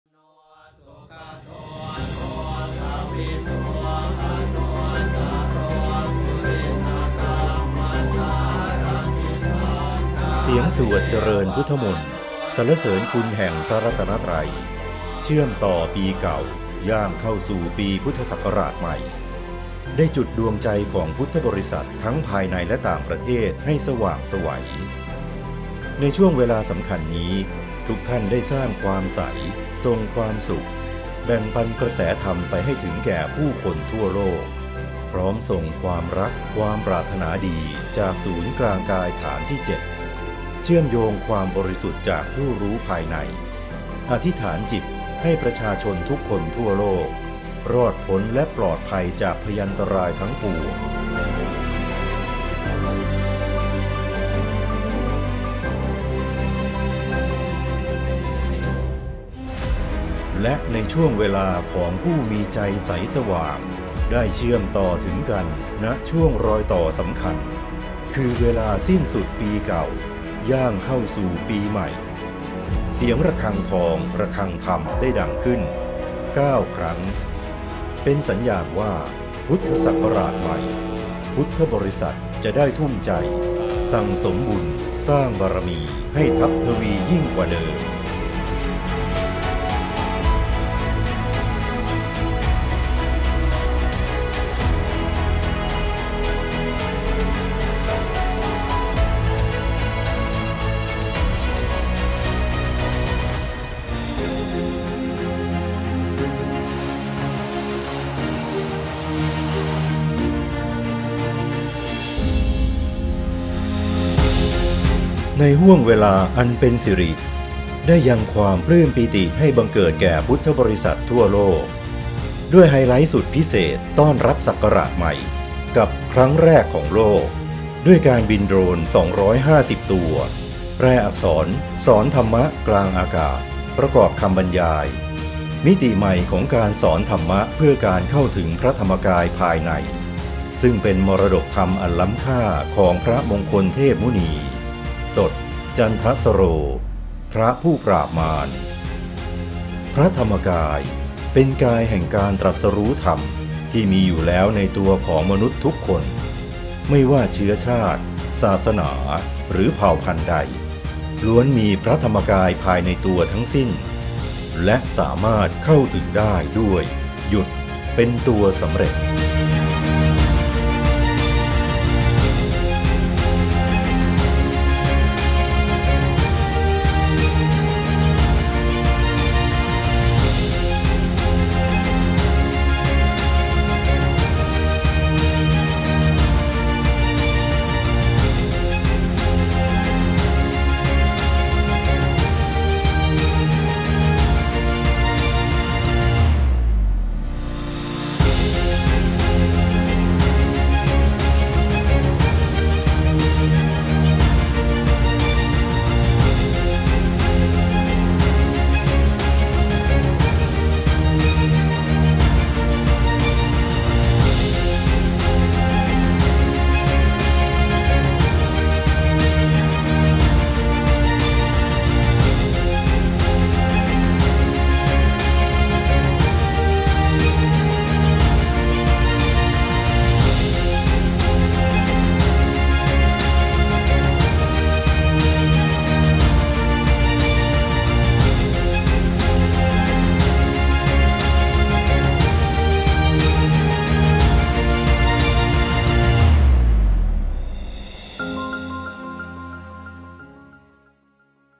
สวดมนต์ข้ามปี 2566 แปรอักษรสอนธรรมะ
สวดมนต์ข้ามปี 2566  ย่ำระฆังรับปีใหม่ 9 ครั้ง
กิจกรรมโดรนแปรอักษรสอนธรรมะ
วันเสาร์ที่ 31 ธันวาคม พ.ศ.2565
วัดพระธรรมกาย จ.ปทุมธานี